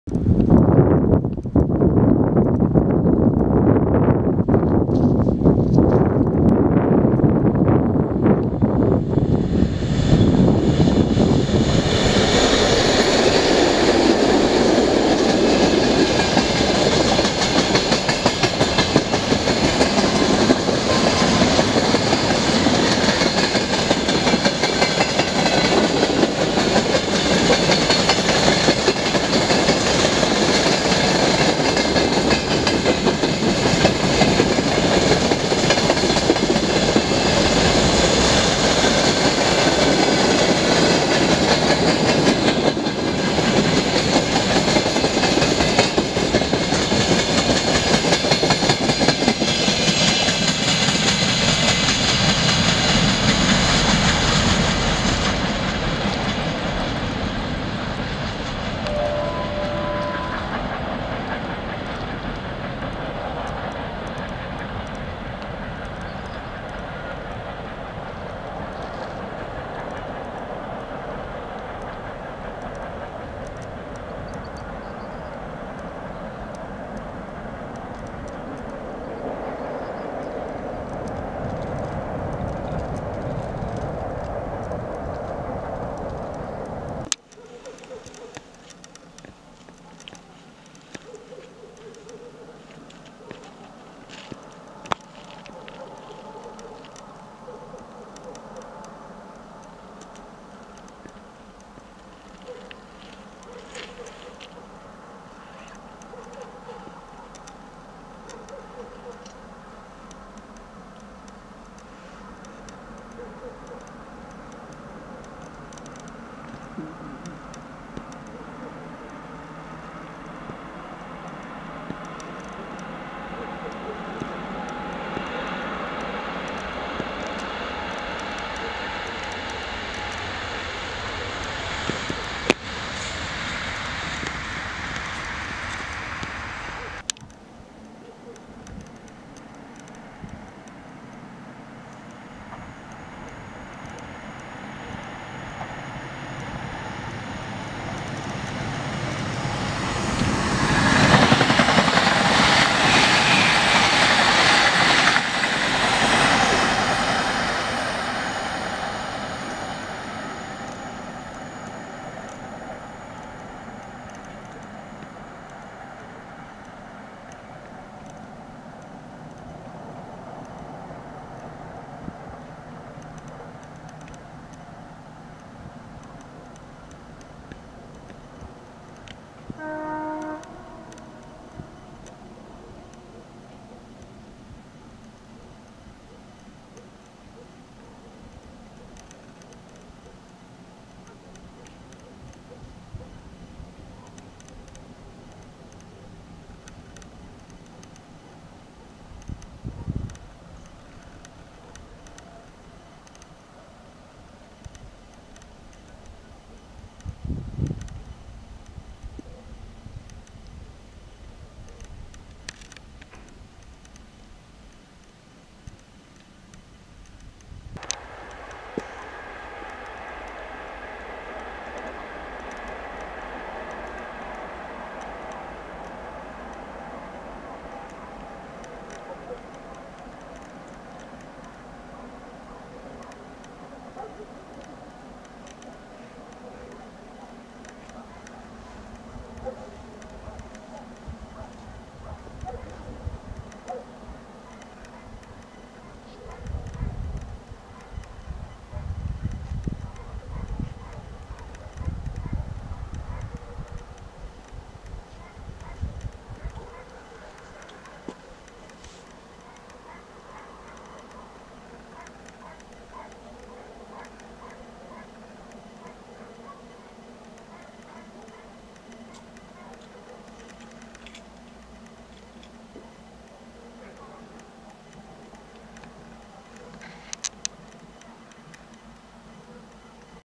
Niedzielne popołudnie z koleją
Na filmie kilka pociągów nagranych w Kozłowie